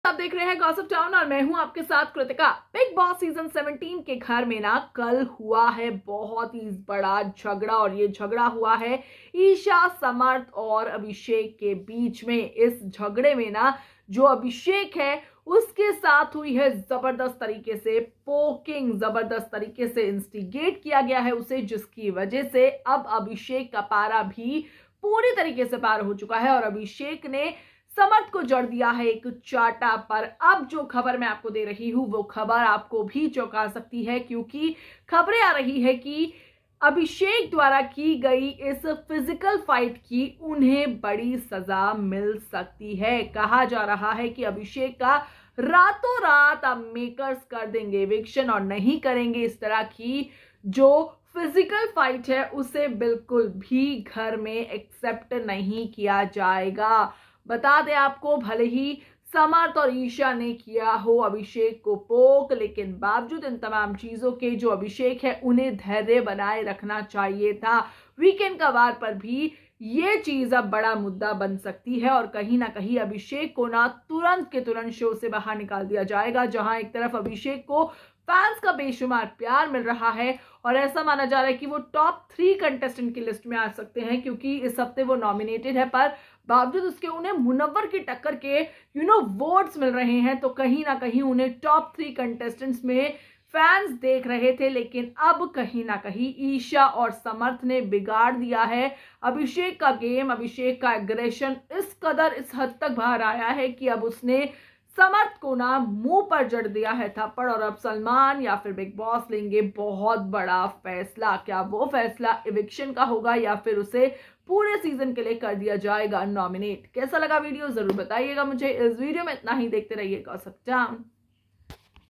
Bigg Boss 17 Shocking Eviction: Abhishek पर Salman लेंगे फैसला! Samarth को थप्पड़ मारना पड़ा भारी?
bigg Boss 17 updates on Abhishek Kumar thappad kand. Watch the video to know more about this.